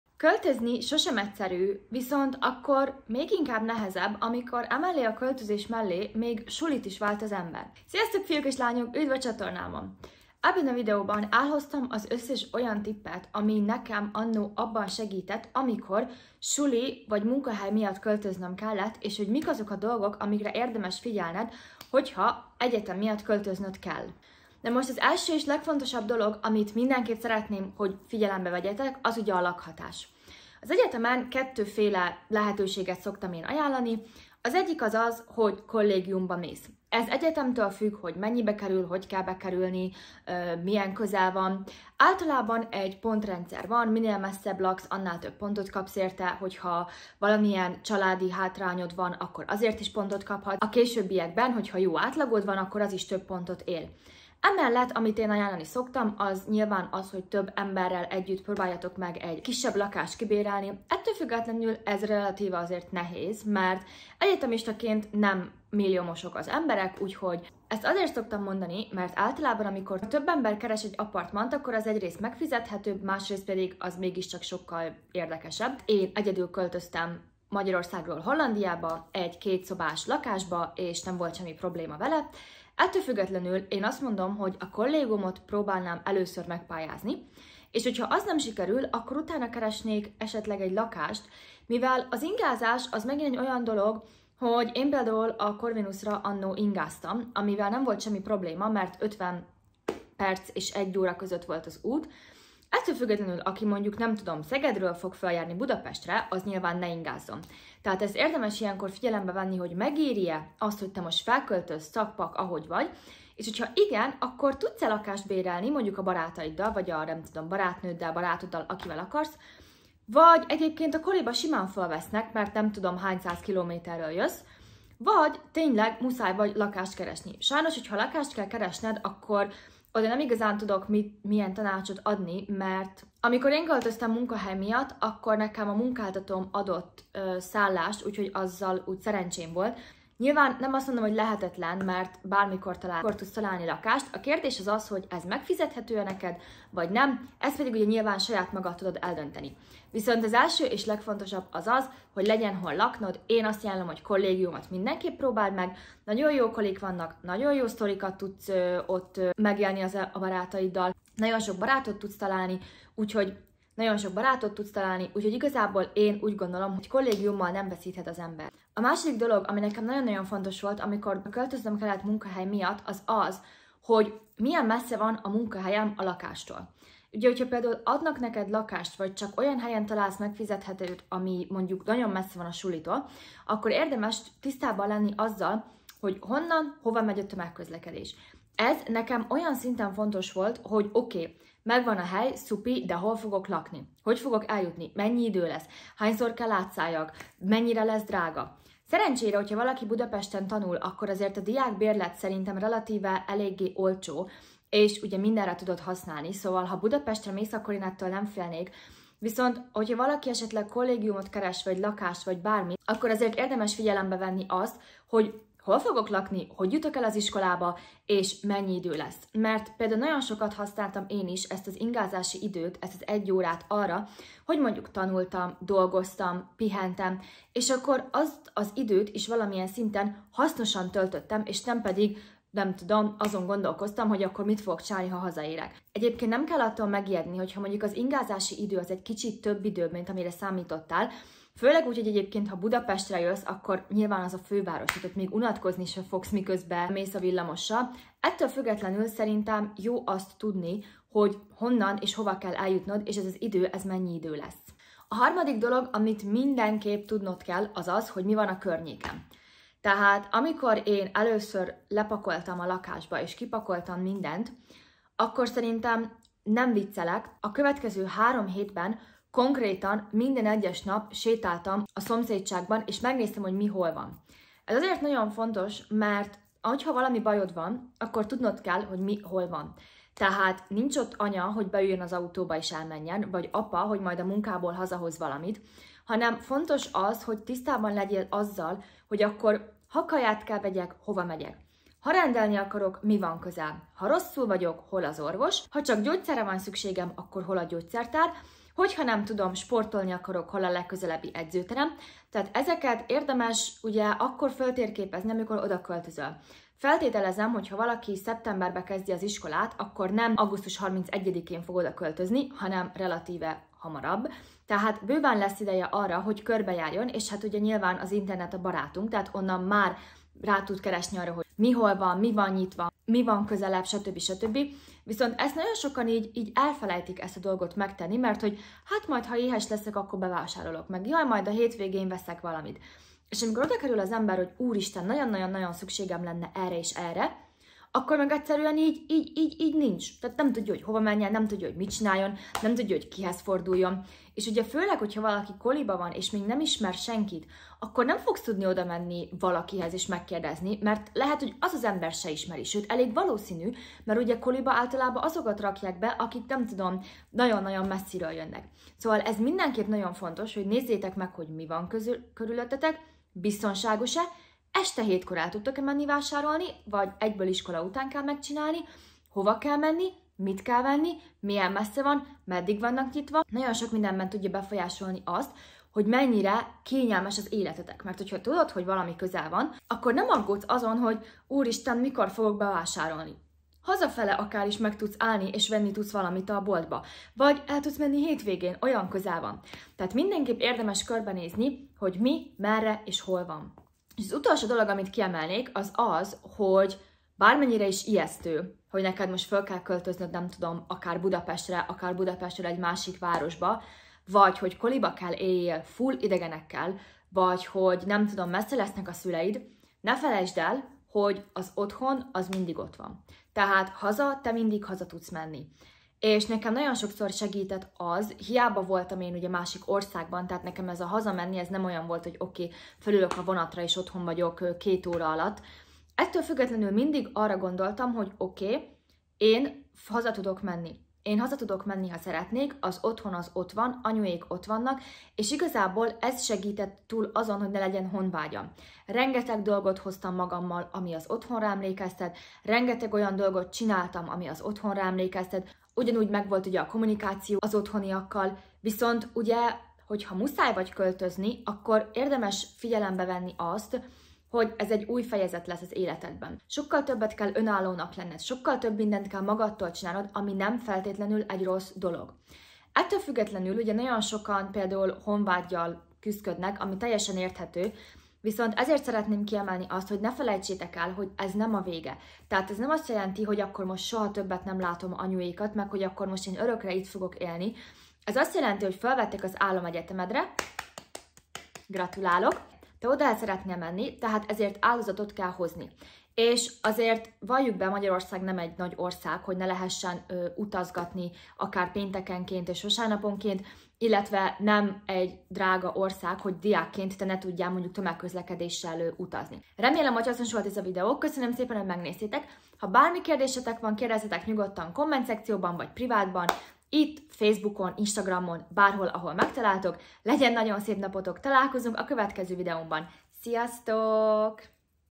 KÖLTÖZZÜNK: MINDEN AMI A SULIKEZDÉSHEZ KELL (lakás/kollégium, tömegközlekedés, bevásárlás,honvágy)
Mit kell tudni akkor, amikor egyetem miatt költözik az ember? Mik a legfontosabb szempontok? Ebben a videóban 4 fontos dolgot beszélünk át: lakás vagy kollégium, tömegközlekedés, bevásárlás és a honvágy.